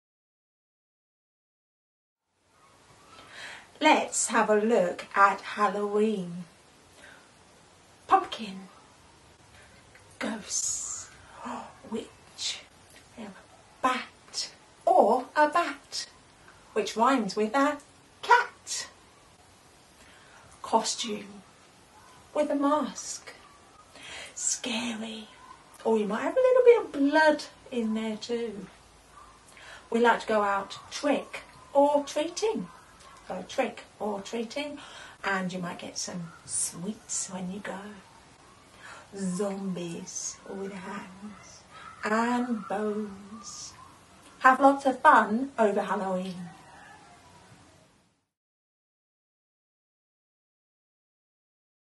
Halloween Signs using BSL Signs
Learn how to sign various Halloween signs in preparation for the Halloween season.